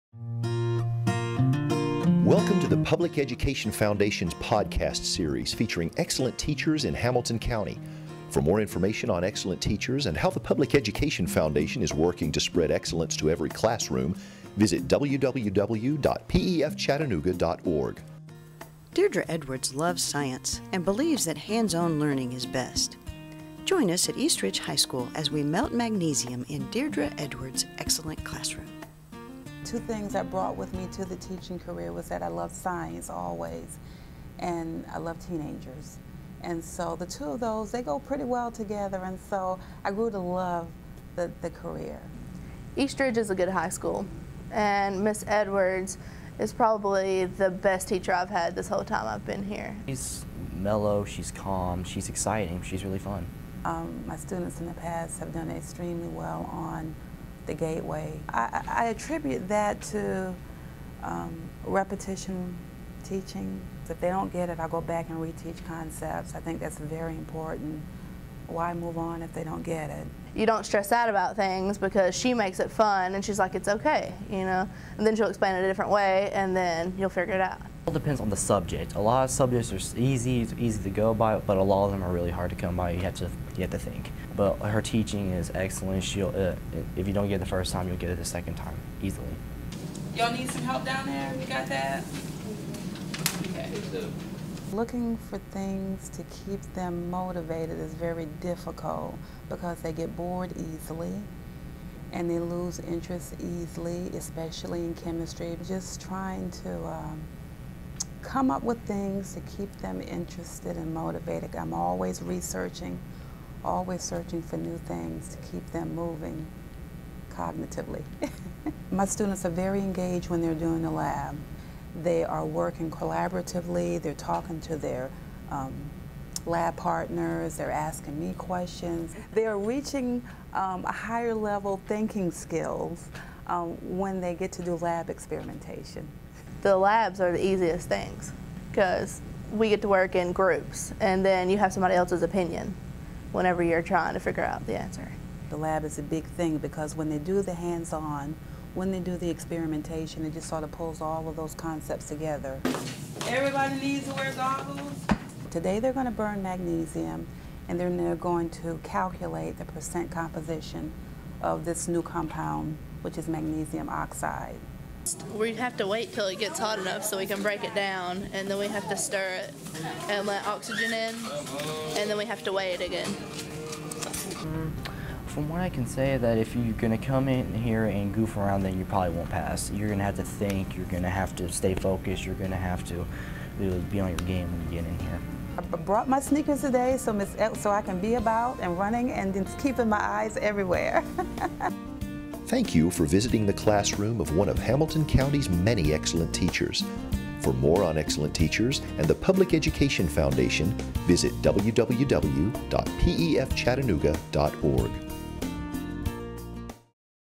Deidre Edwards' Excellent Classsroom
East Ridge High School science teacher Deirdre Edwards believe in hands-on learning.